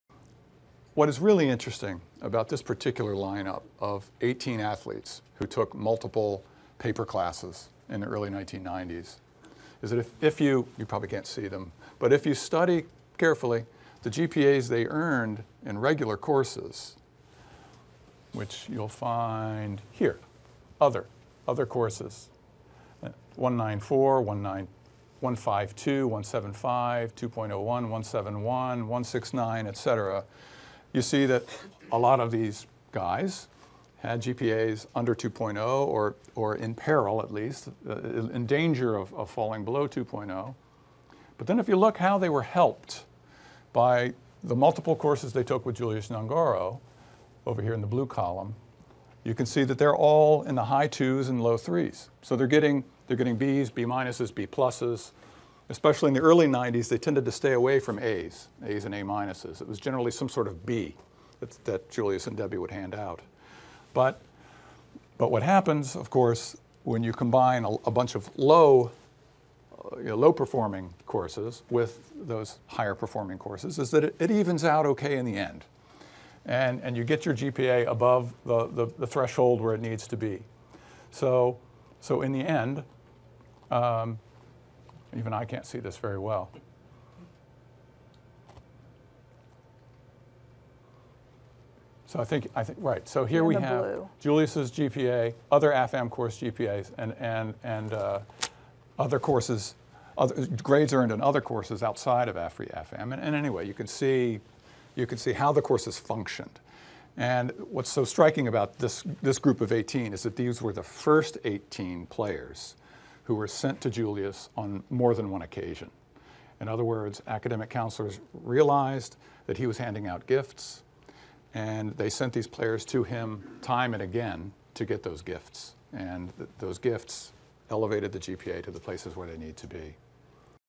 Jay Smith & Mary Willingham, "Cheated"
Jay Smith, history professor at the University of North Carolina, and Mary Willingham, a former employee at the Center for Student Success & Academic Counseling at UNC-Chapel Hill , report on two decades of academic fraud at the University of North Carolina, where student-athletes were given fake or "paper classes" to artificially boost their GPA's.   The authors examine the inner-workings of this fraudulent system, the role University officials played, and the $16 billion college sports industry.  Jay Smith and Mary Willingham speak at Quail Ridge Books & Music in Raleigh, North Carolina.